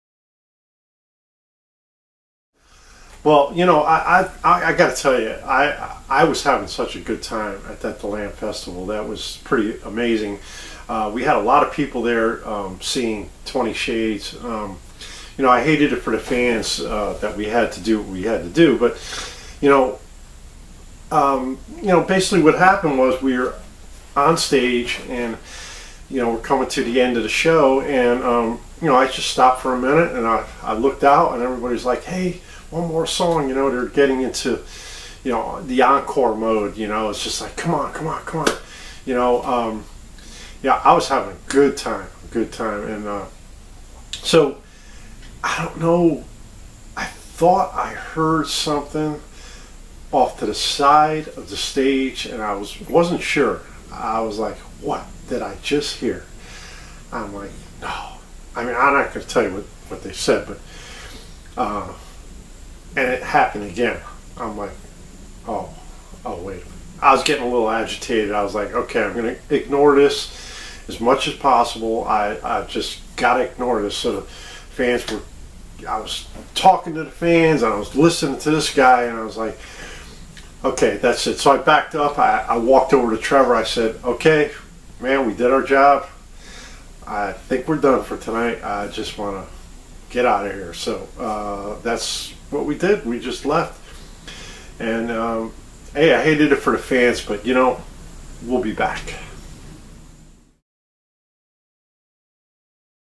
James Pergolatti Interview Deland fl Music festival
James Pergolatti talks about fan getting out of control at Deland fl music Festival